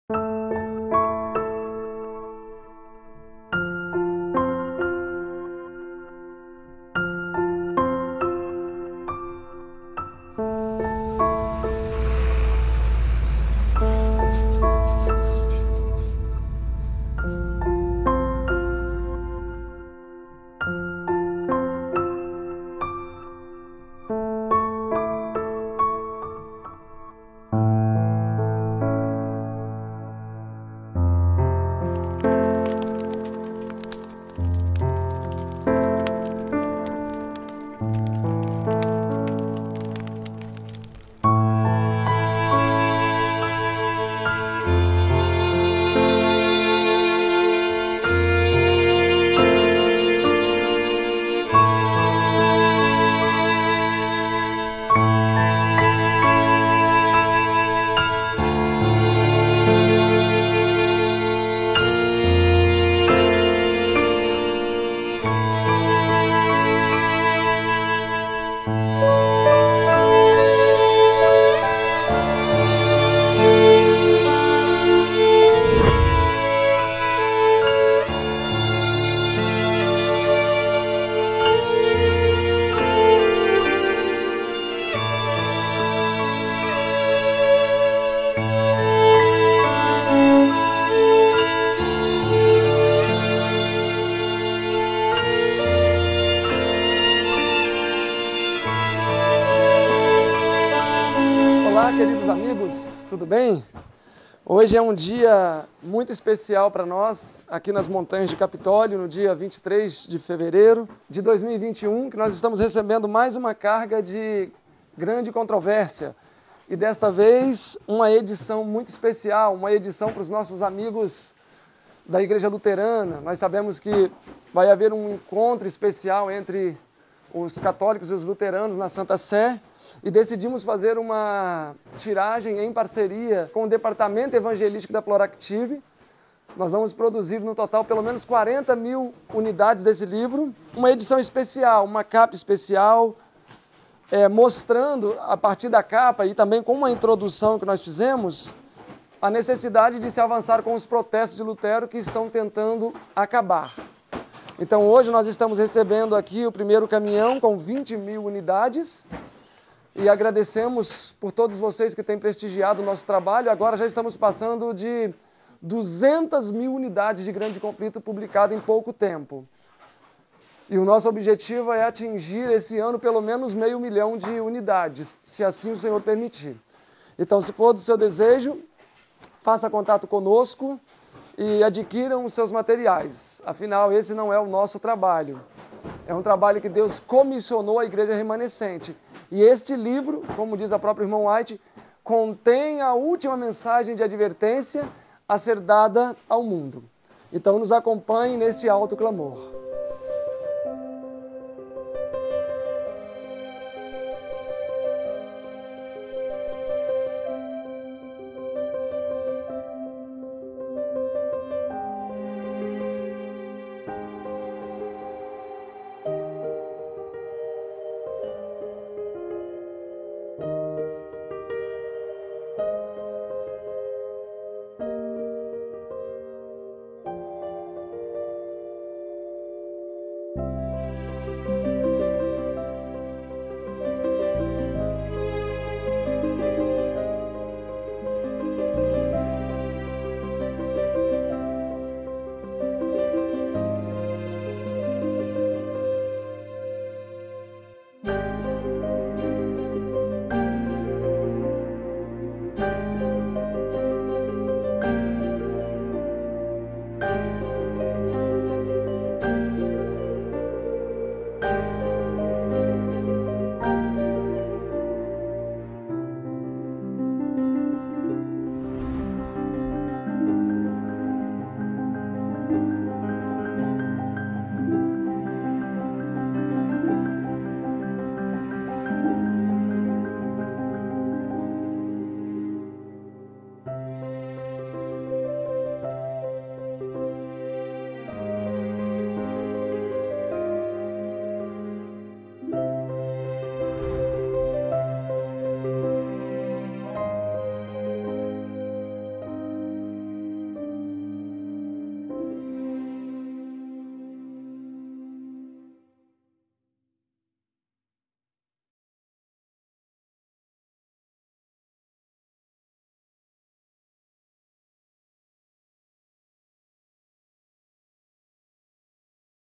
(350) - GRANDE CONTROVÉRSIA - PROJETO ALTO CLAMOR - 20 MIL JÁ PRONTOS !!
Registramos aqui os primeiros 20 mil A Grande Controvérsia do projeto Alto Clamor que foram  entregues no dia 24/02.
Esperamos e contamos com seu apoio neste projeto para alcançarmos o povo Luterano e lembrá-los de que a luz da reforma jamais se apagará !!

Doações..

CRÉDITOS:
Apresentação: Silverino Küll
Produção executiva: Silverino Küll
Imagens, Direção e Edição: Raphael Aguiar